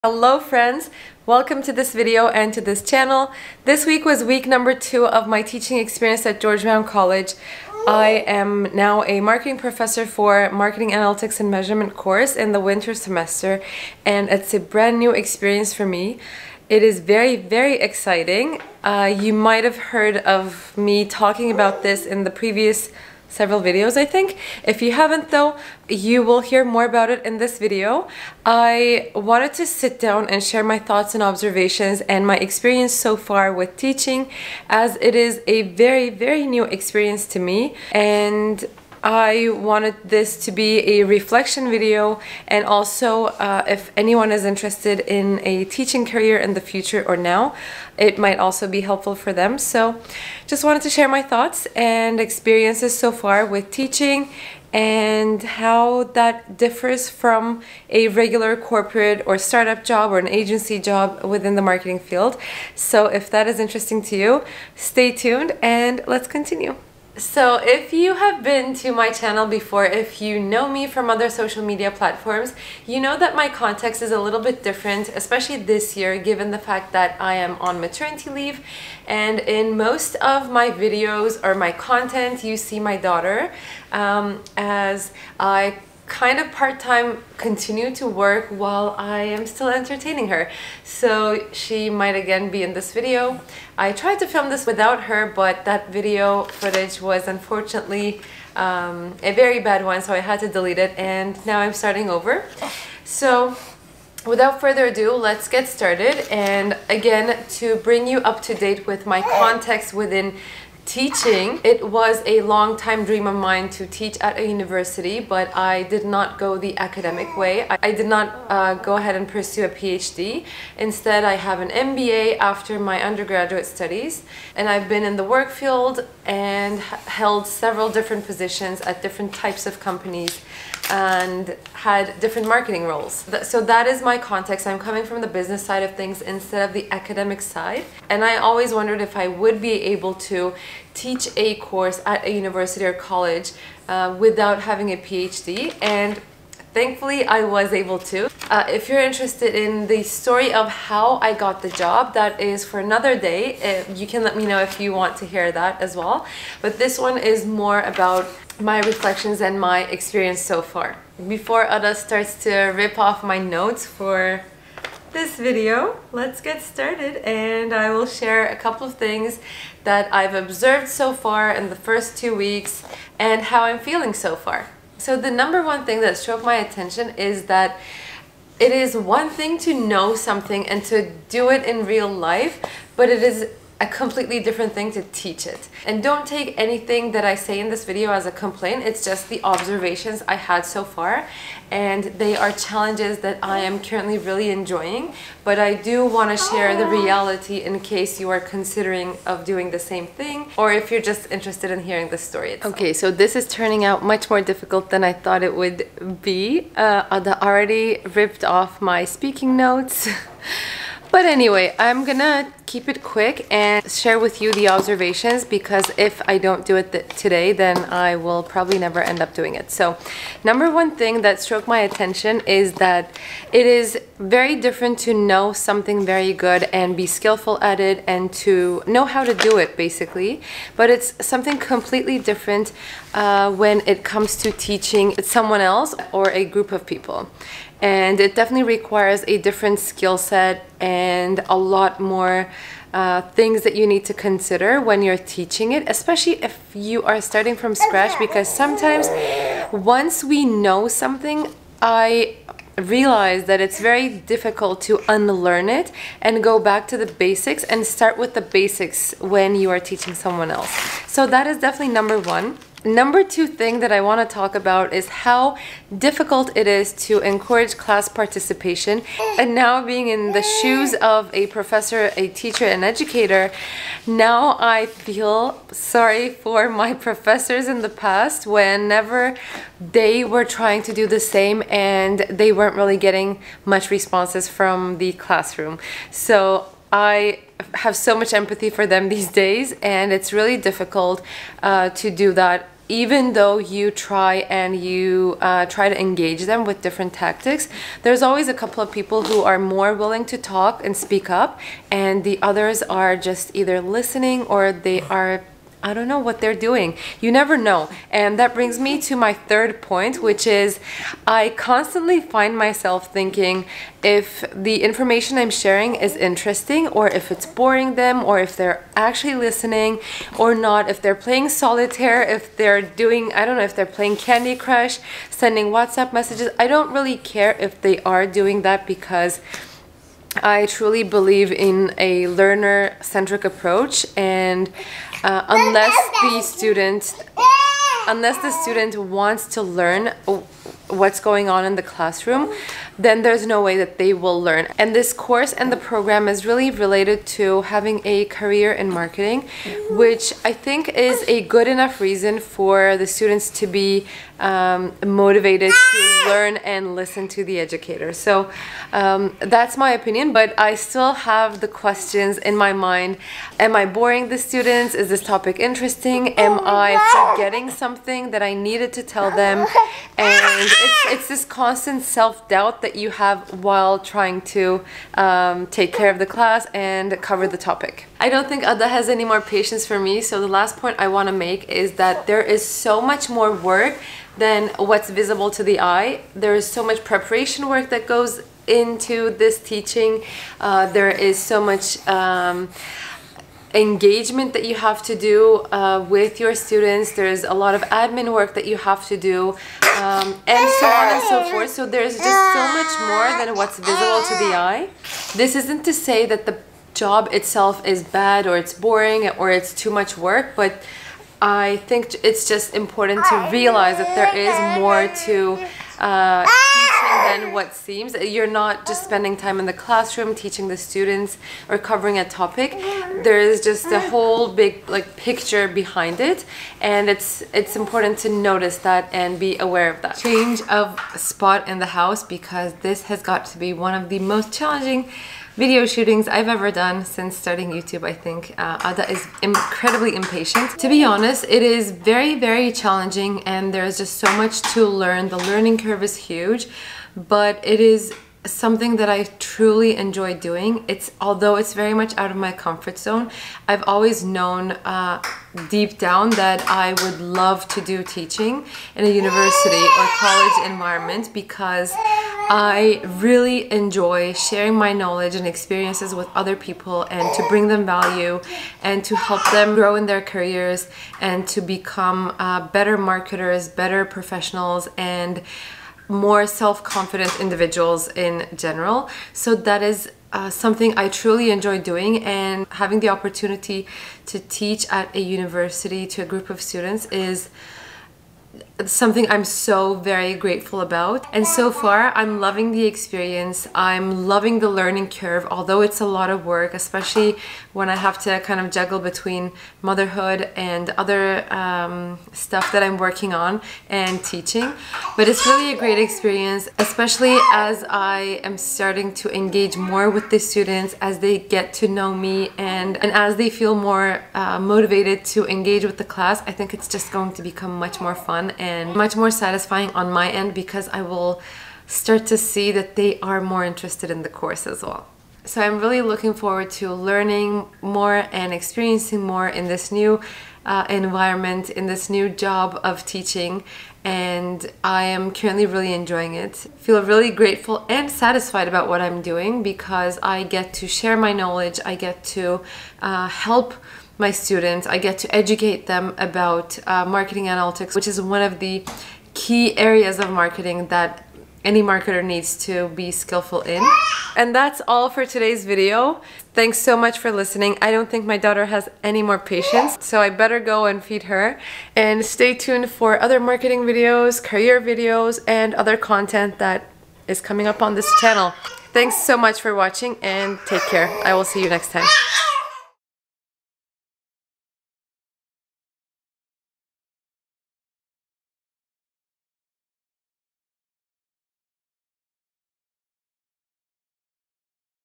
ALTERNATIVE MARKETING CAREER - Experience as a Marketing Professor 👩🏻‍🏫
Having a career in marketing doesn't always mean you work in a marketing team of a company or work with in an agency setting. What could be an alternative marketing career?

Recently I've had a twist in my marketing career when I started working as a marketing professor at George Brown College, where I teach a marketing analytics course this semester. In this video, I shared my experiences and observations as a first time marketing professor. You'll hear my thoughts from the first two weeks and I hope these experiences are insightful for anyone who considers this option.

▷ CAMERA GEAR:
○ My camera
○ Vlog camera
○ Mini tripod

Browse & shop my favorite gadgets and items I use on a daily basis ➳